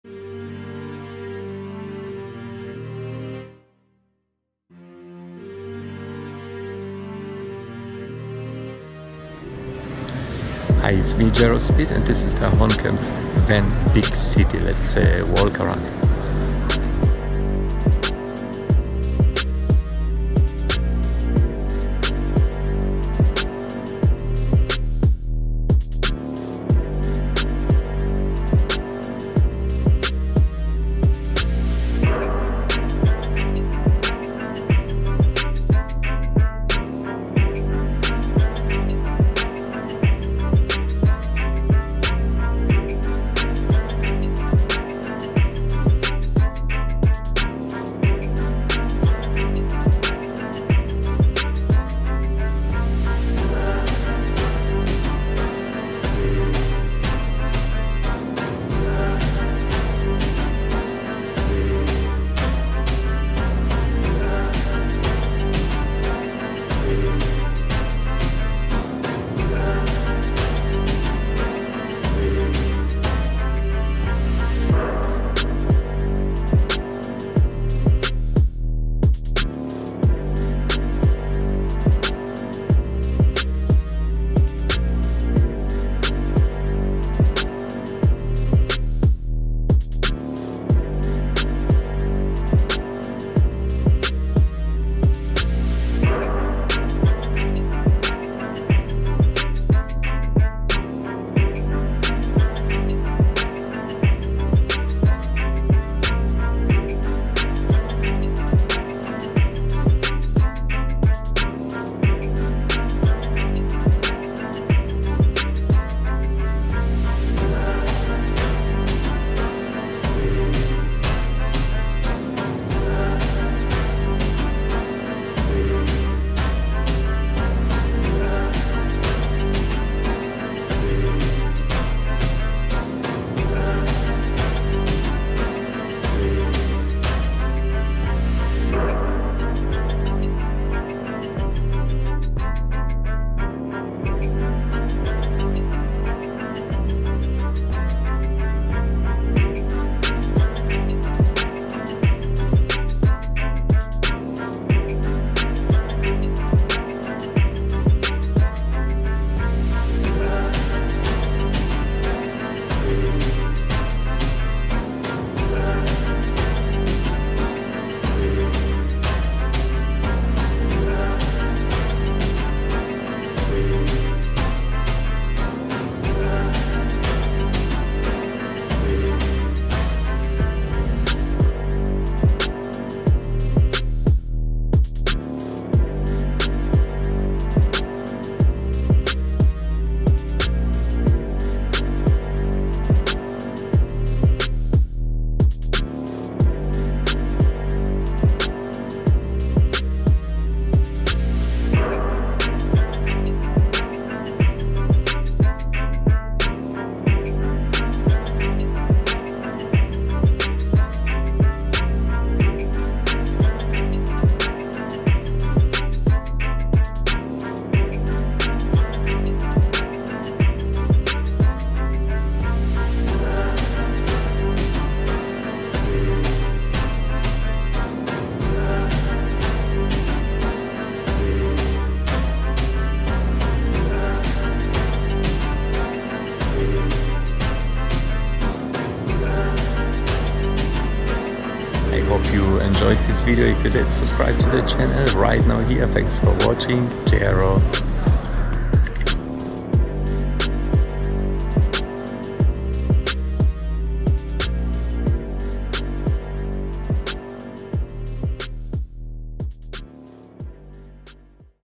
Ahorn Camp Van Big City RV Camper Van Renault Trafic Camping car walkaround and interior V0662
jaro speed
Ahorn Camp Van Big City
Renault Trafic 2,0L / 110kw / 150hp
L:540  / W:196 / H:203 / weight: 3.050kg
total price of exhibited vehicle in this configuration on this exhibition was: 70.064.-EUR
model 2023